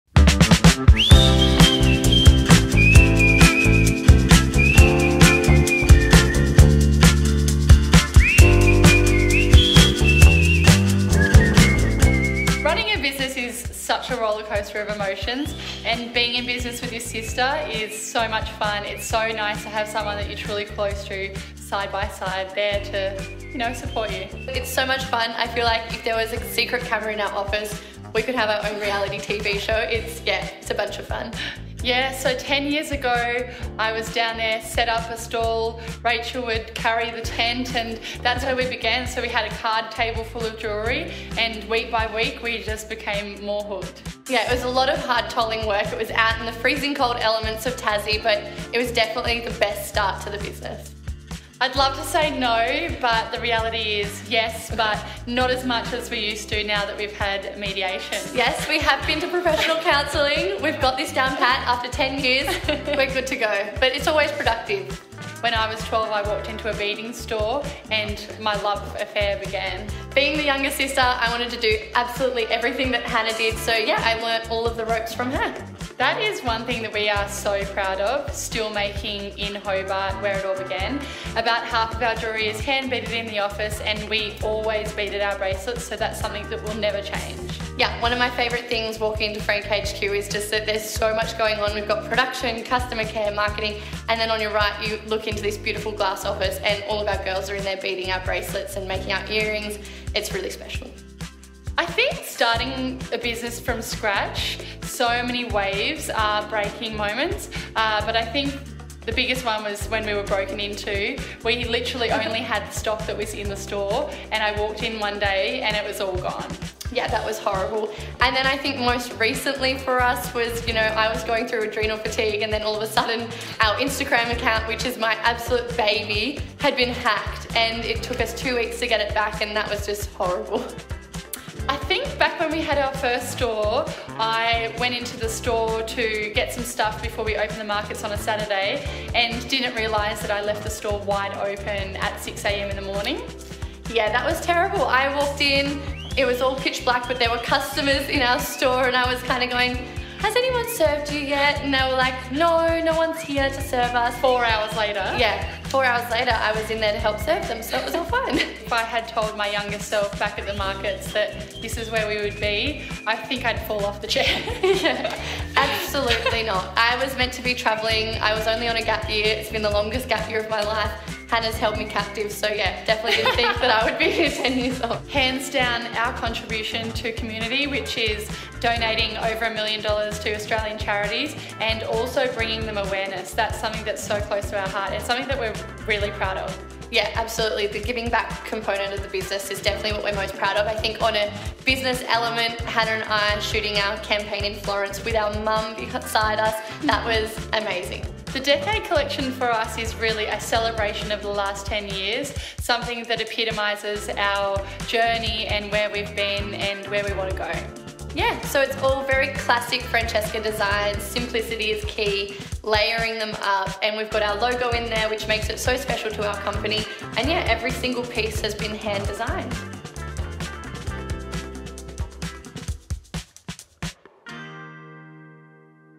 A Decade of Francesca 💫
Celebrating 10 years of Francesca! We asked you for your questions, so here are the top 10! Thank you from the bottom of our hearts for being a part of this incredible journey. We hope you love this collection as much as we do. xx